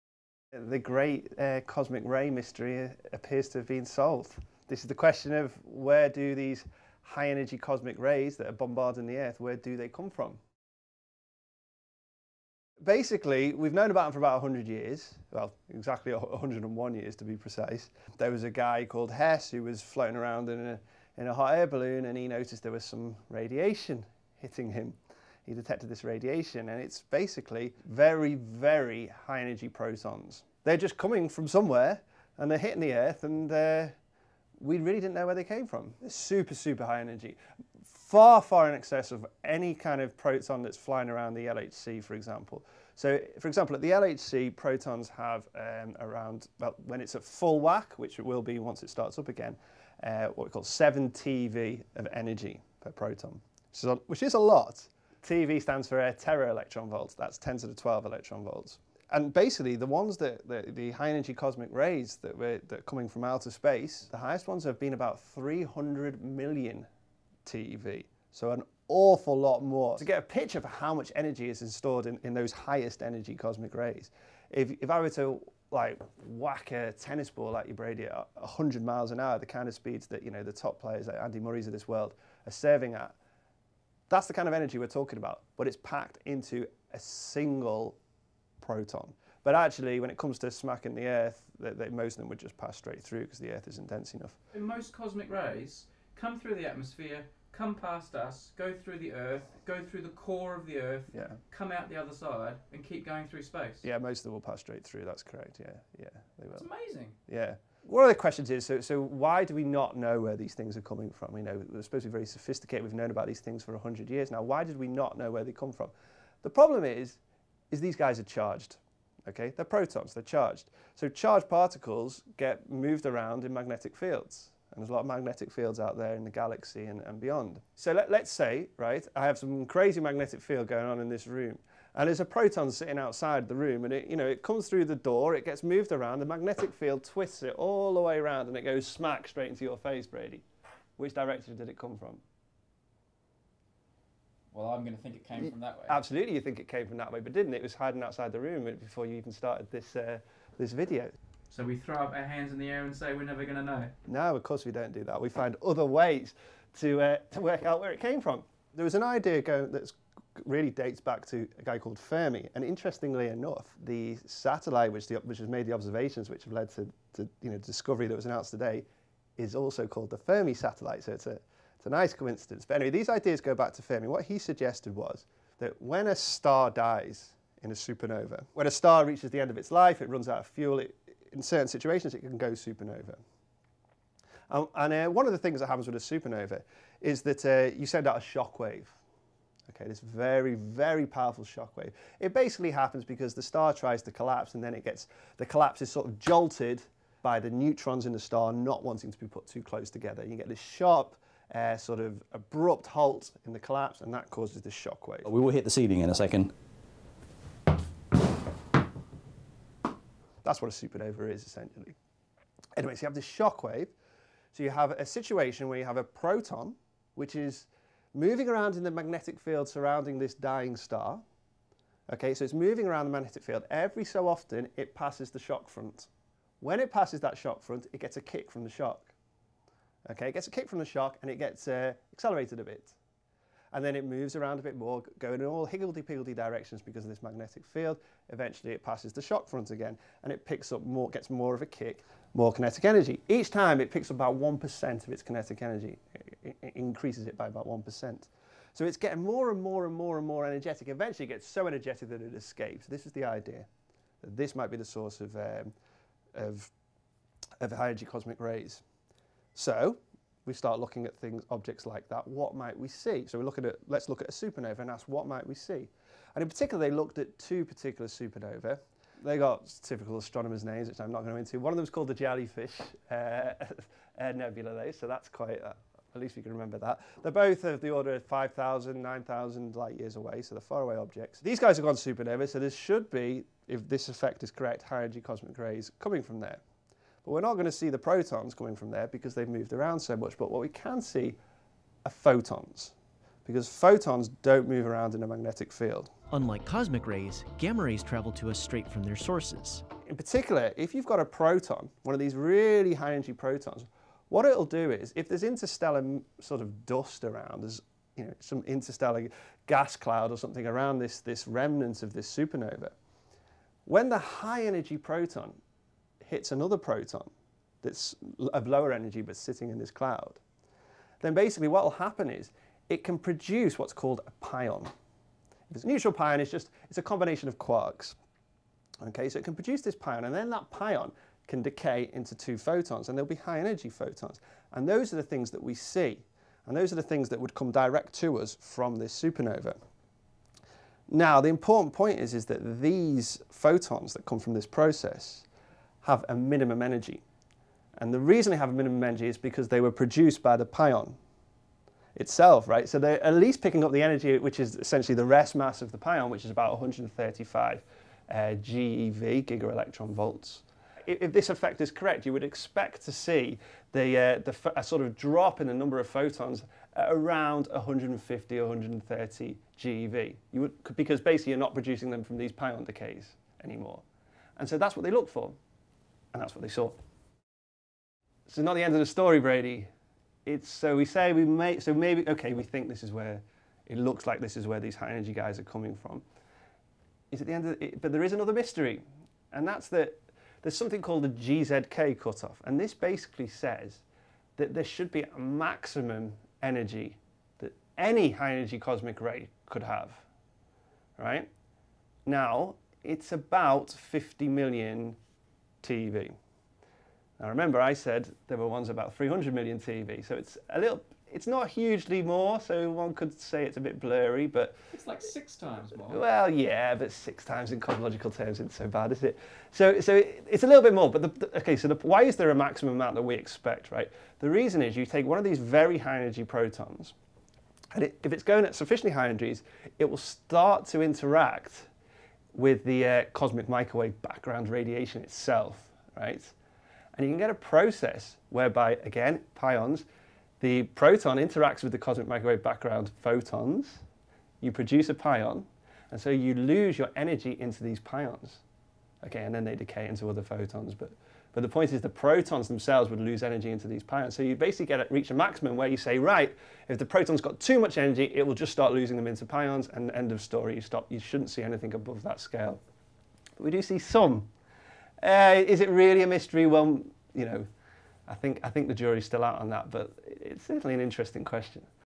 Cosmic Rays Mystery Solved - Sixty Symbols
New observations seem to settle the question of where cosmic rays come from.

Dr Tony Padilla discusses the research.

COMMENT FROM TONY: "My comments at 1.30 were a bit hasty. Cosmic rays do not pass through the earth. Most collide in the upper atmosphere. I was actually thinking of the LHC safety debate. If they exist, mini blackholes produced by cosmic ray collisions on earth would slip straight through because the earth isn't dense enough. But not the protons themselves."

(lots of good science news, links and jokes mixed with some over emotional football commentary)